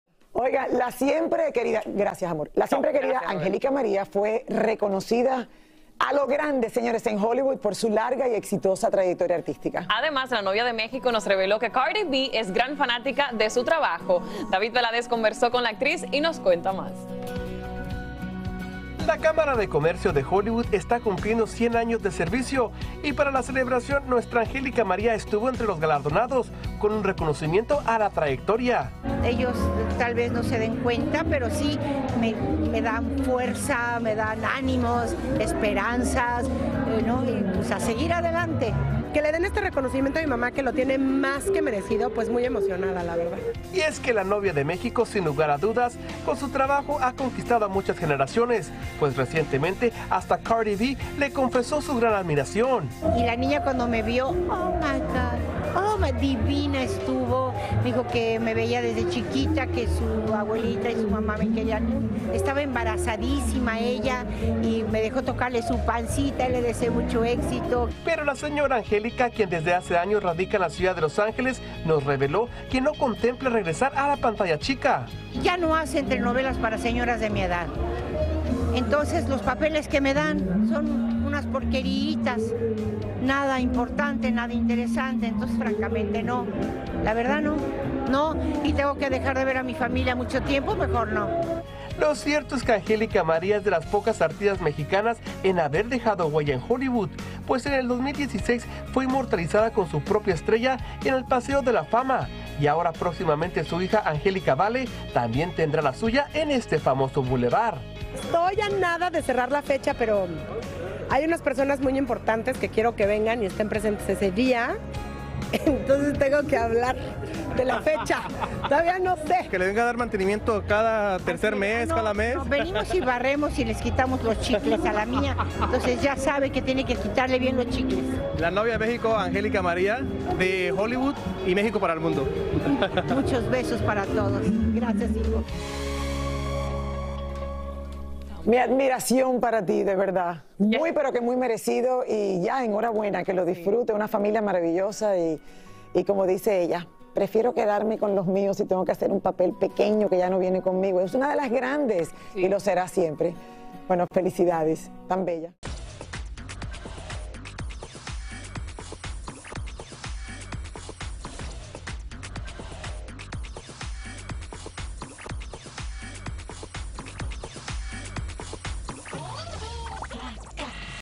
Angélica María recuerda cuando Cardi B le expresó toda su admiración al conocerla | GYF
La 'novia de México' fue reconocida por la Cámara de Comercio de Hollywood por su trayectoria artística, en dicho evento, logramos hablar con la primera actriz, quien no contempla volver a la pantalla chica. Por su parte, su hija Angélica Vale nos adelantó cómo va el proceso para recibir la estrella en el Paseo de la Fama donde su madre ya tiene la suya.